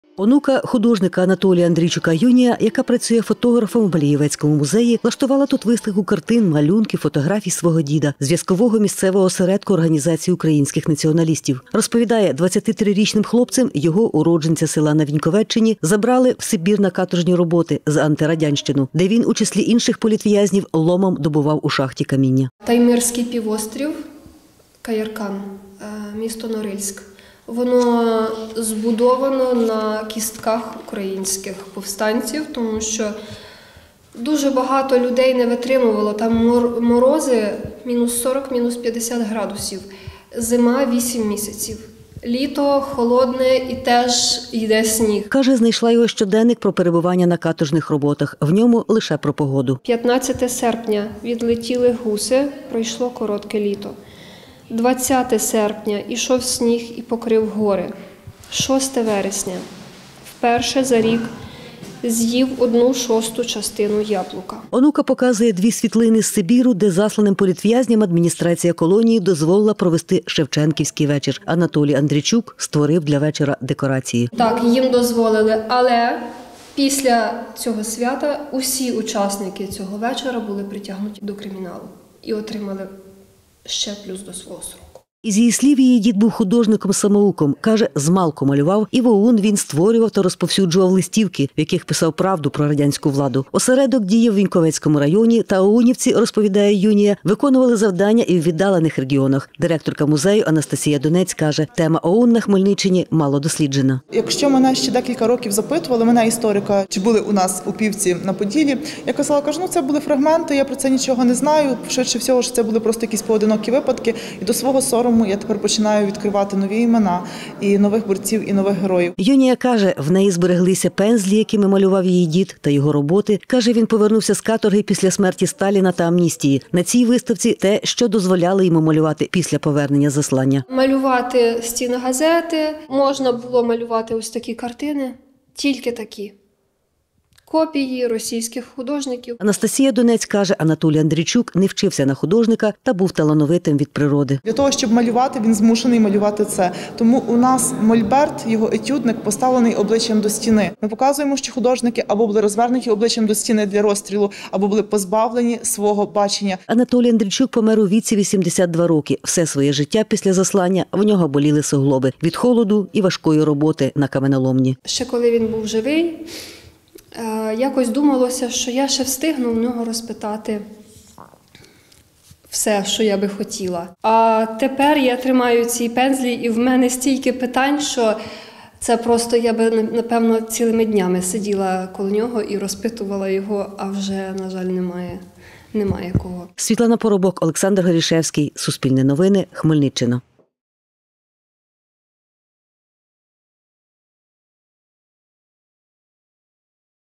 В одному з музеїв Хмельниччини онука відкрила біографічну виставку зв'язківця ОУН
25 років Сибіру за антирадянщину отримав художник, зв’язковий місцевого осередку Організації українських націоналістів Анатолій Андрійчук, уродженець села Гоголі Віньковецького району. Юнаком він вступив до ОУН, щоби писати та розповсюджувати листівки про винищування радянською владою селян як цілого класу суспільства в надії, що народ повстане проти поневолювачів. Зараз онука Анатолія Андрійчука влаштувала в Малієвецькому музеї його виставку.